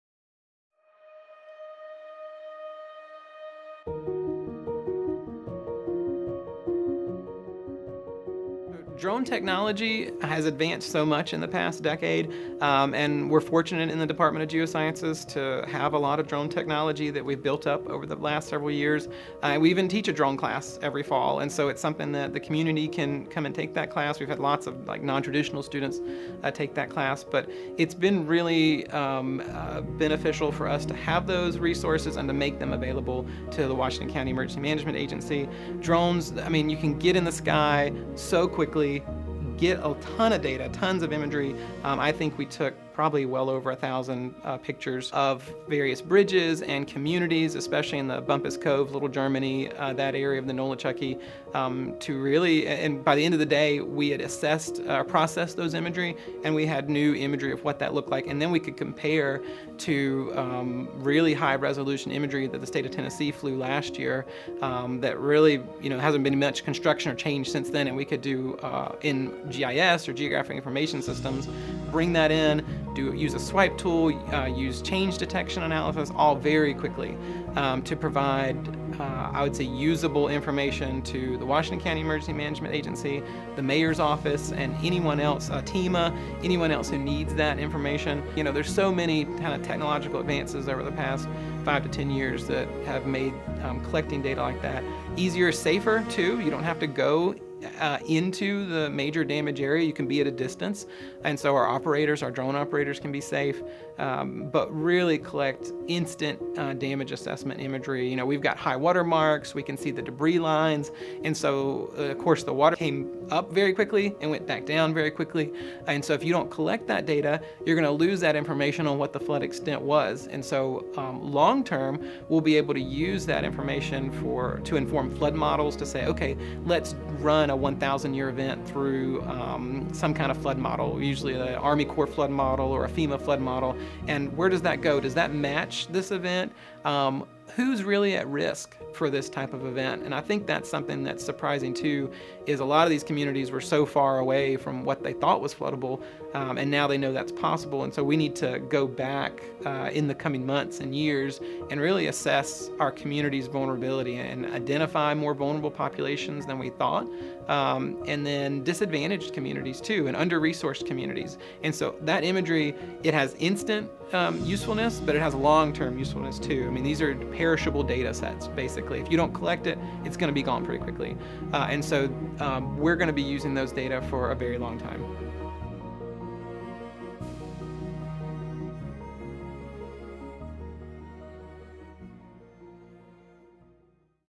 Using Drone Technology to Map Hurricane Helene’s Damage
Discover how drone technology played a crucial role in mapping the widespread flood damage caused by Hurricane Helene.

In this episode of Rising with Hope: Appalachia After Hurricane Helene, Dr. Andrew Joyner from ETSU’s Department of Geosciences explains how drones provided rapid imagery to help emergency responders and local agencies, including the Washington County Emergency Management Agency and Tennessee Emergency Management Agency.

With thousands of photos captured, this cutting-edge technology offered instant flood data and will be used for years to better predict future flood events and protect vulnerable communities. Watch how these advancements help keep communities safer and better prepared for extreme weather events, and allow for long-term analysis to model future flood risks.

The full series is available on ETSU's YouTube

Bucs Rebuild Together:  Bucs Rebuild Together | ETSU's and ETSU Health's Response to Hurricane Helene